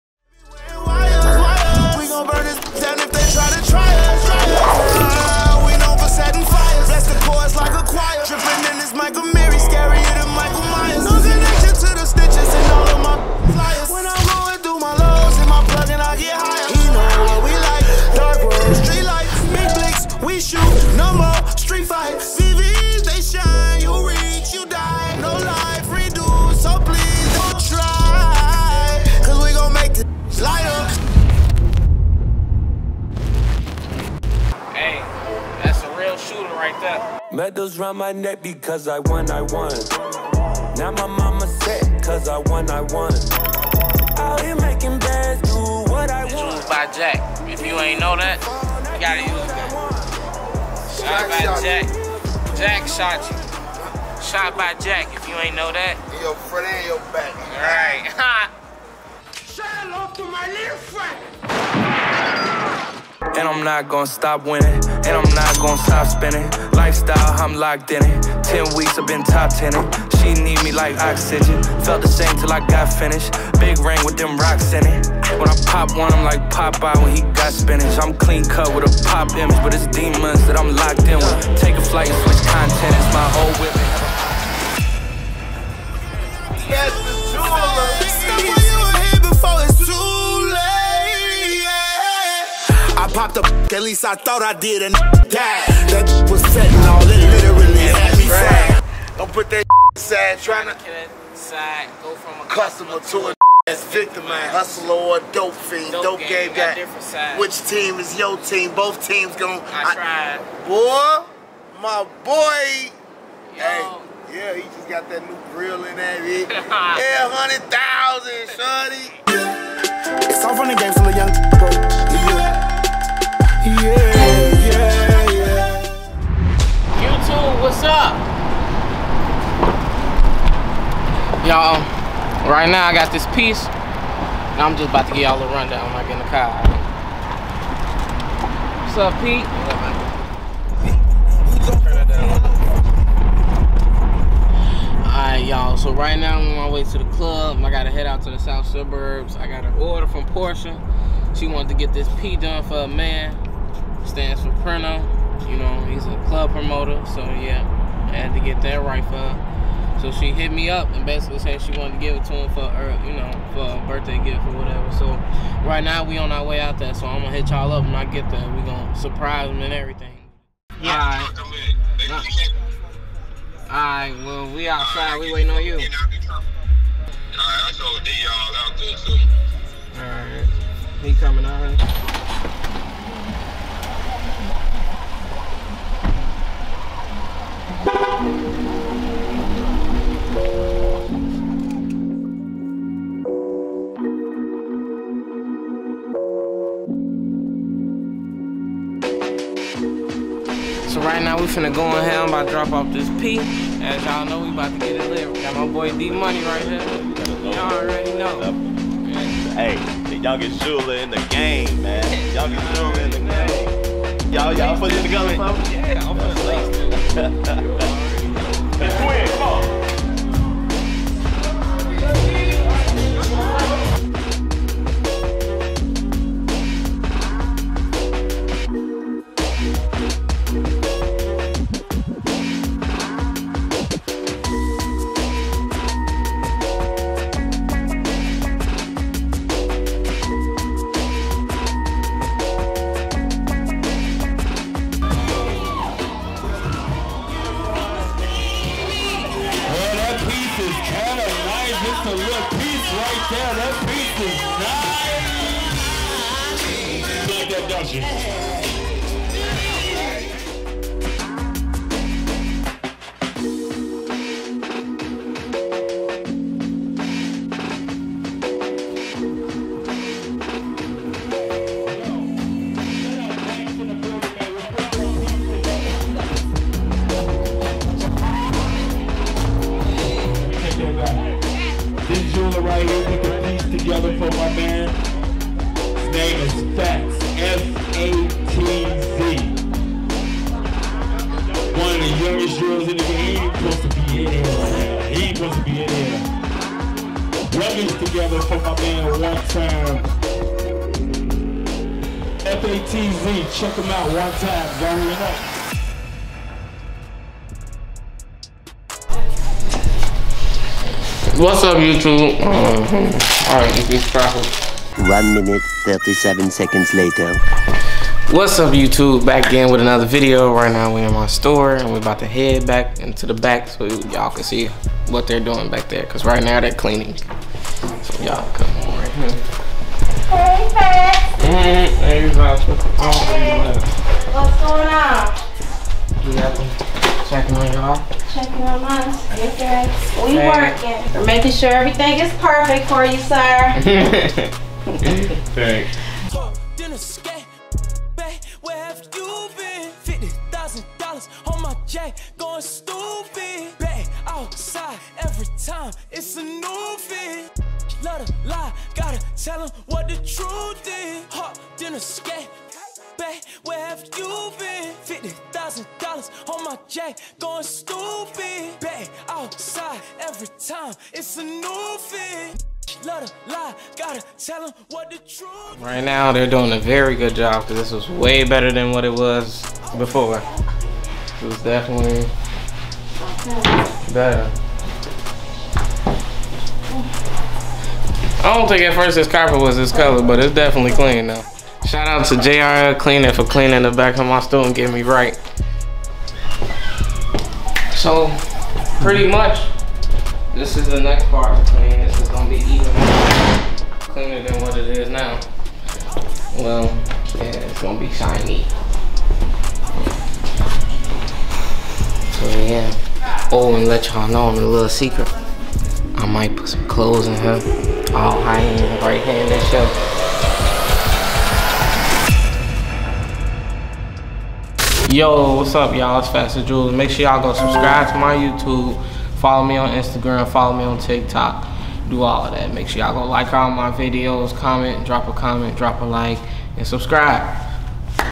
Present for Prino
FATZ The Jeweler pulls up to a south side suburbs club to surprise Prino with a custom P piece that his girlfriend ordered him for his birthday.